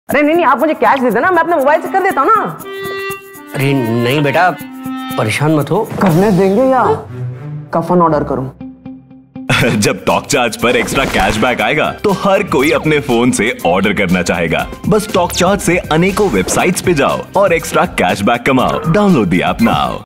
Earn Extra Cashback with TalkCharge | Transfer Cashback to Bank Account
All these ages we have been sniffing off for the extra cashback. Are you ready for a journey from 'Guaranteehaikya?' to 'GuaranteehaiwithTalkCharge'? TalkCharge is a Cashback & Coupon App/Website helping users to earn cashback as Real Money on the online transactions you make while spiralling through your favourite brands. India, get ready to earn while you spend with TalkCharge's guaranteed Cashback.

1. Login/Register at TalkCharge
2. Search for your store/website/merchant from where you want to shop
3. You will see an Orange Colour ActivateCashback button; you can directly click to it and get redirected to the Merchant Website;
OR

Look out for various Deals and Coupons present at TalkCharge Store Page and click on ActivateCashback button right next to your loved deals, so as we will directly redirect you to that particular page.

* Now simply shop and pay using any payment mode you want, just because you have Shopped Via TalkCharge, we will give you your GuaranteedCashback in TalkCharge Wallet or even your own Bank Account!

* Also for your assistance TalkCharge Wallet Money can further be used to Recharge, Pay Bills or Purchase GiftCards from the website

Shop Via TalkCharge Now!!

Follow us to stay updated with all the upcoming offers to help you save while you spend